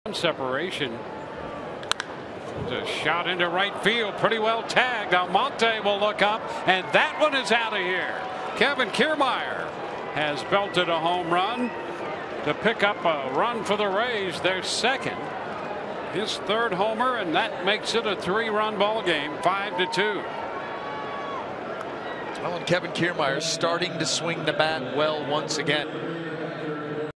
TB@CLE: Kiermaier crushes a solo home run to right
Kevin Kiermaier launches a solo home run to right field, bringing the Rays within three runs in the top of the 2nd inning